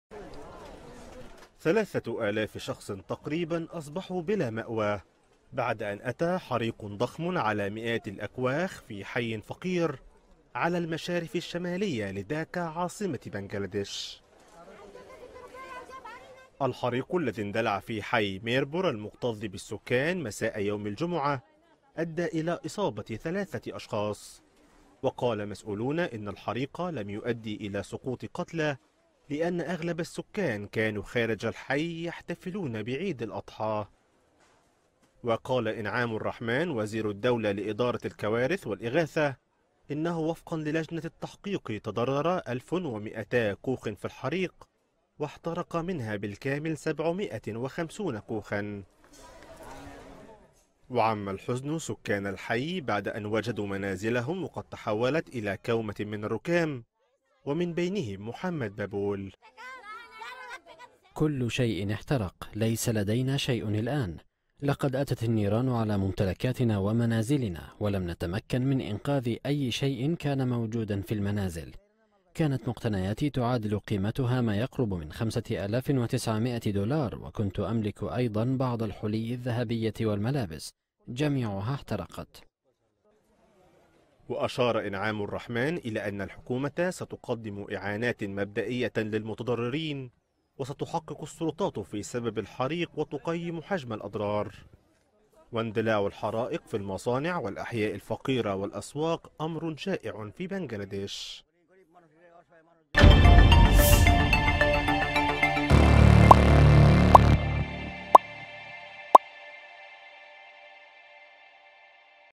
حريق بحي فقير على مشارف داكا يشرد الآلاف
قال مسؤولون في حكومة بنجلادش إن نحو ثلاثة آلاف شخص أصبحوا بلا مأوى بعد أن أتى حريق ضخم على مئات الأكواخ في حي فقير على المشارف الشمالية للعاصمة داكا.